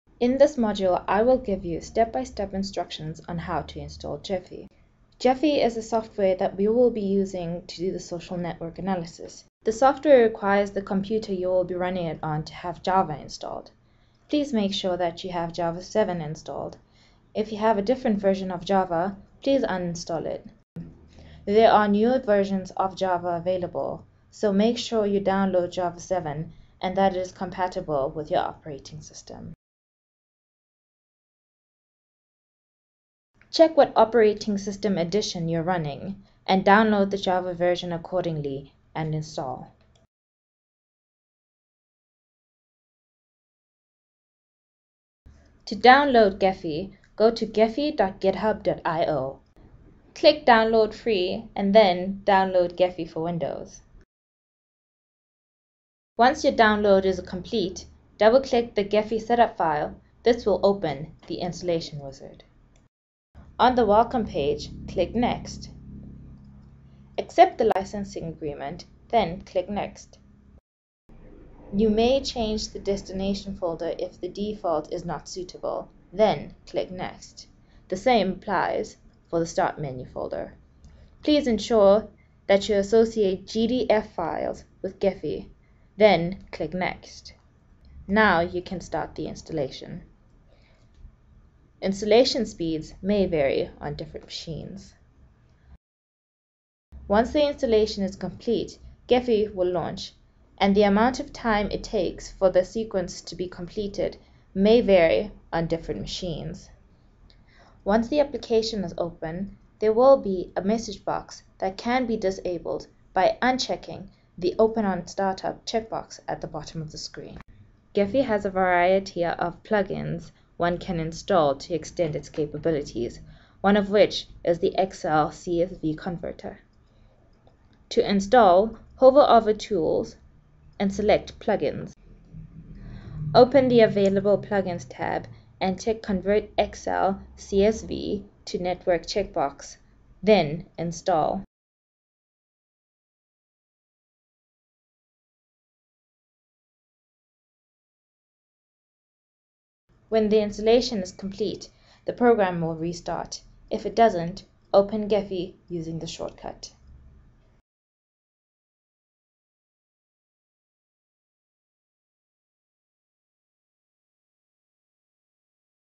Gephi Software Installation
How to install Gephi and excel conversion plugin